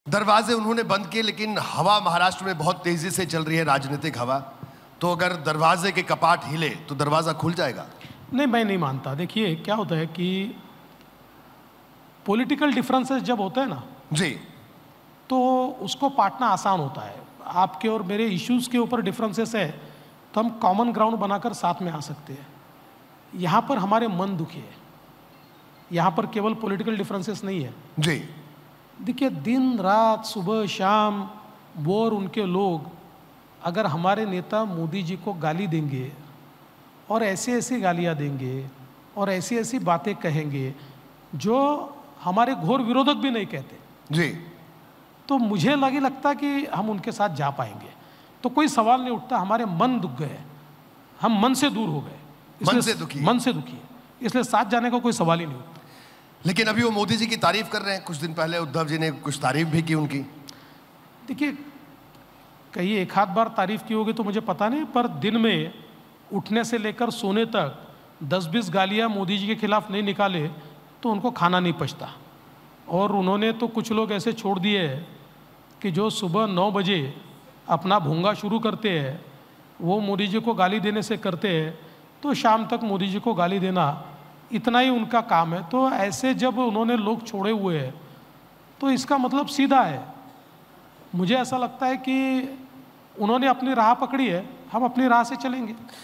Idea Of India Summit 3.0: फडणवीस का छलका दर्द ! 'उद्धव जी ने हमारा मन दुखाया है' | EXCLUSIVE | ABP
Ideas of India 2024 में महाराष्ट्र के उपमुख्यमंत्री देवेन्द्र फडणवीस ने महाराष्ट्र और देश के कई राजनैतिक मुद्दों पर बात की है. हाल ही में महाराष्ट्र के कई दिग्गज नेताओं ने विपक्षी पार्टियों से इस्तीफा दिया था. जिसको लेकर देवेन्द्र फडणवीस ने बीजेपी और महाराष्ट्र सरकार का विजन साफ किया है. तो वहीं फडणवीस ने उद्धव ठाकरे पर बड़ा बयान दिया है. उन्होंने कहा कि 'उद्धव जी ने हमारा मन दुखाया है..मुझे नहीं लगता की हम वापस उनके साथ जाएंगे'